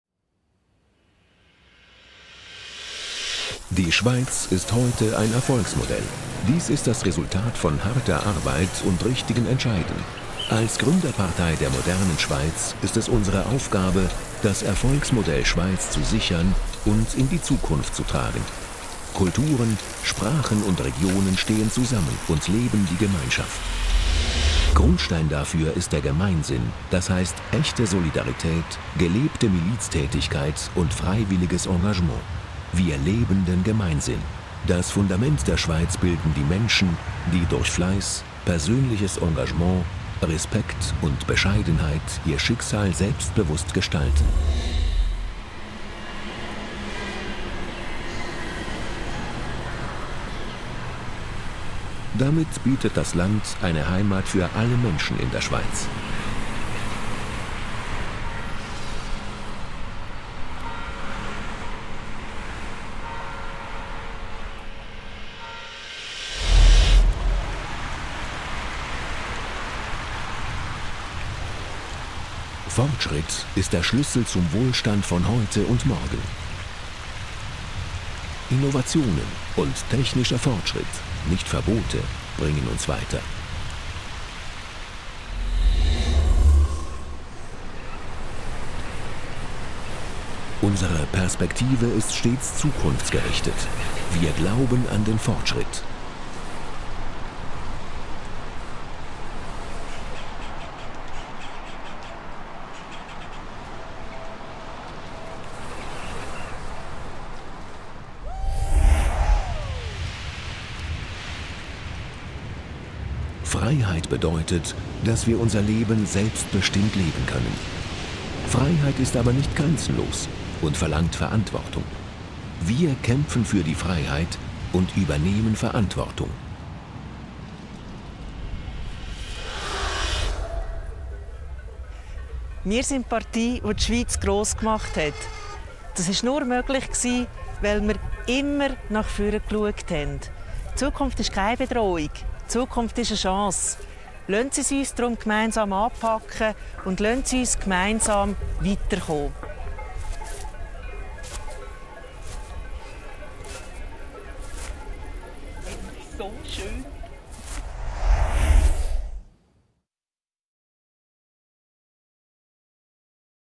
FDP Schweiz 360° experience
"Aus Tradition sind wir von der
FDP modern – und das seit 170
Jahren. Seit Beginn weg prägten
wir die Gesellschaft und legten
den Grundstein für einen starken
Mittelstand."
360grad Video mit Flugaufnahmen über den schönsten Gegenden der Schweiz. Vom Bundeshaus in Bern, ins Emmental, über die Sunnibrücke bei Klosters, über die Stadt Zürich, einen Flug mit den Paraglidern über dem Vierwalstättersee und einen Rundblick vom Buochserhorn mit Petra Gössi.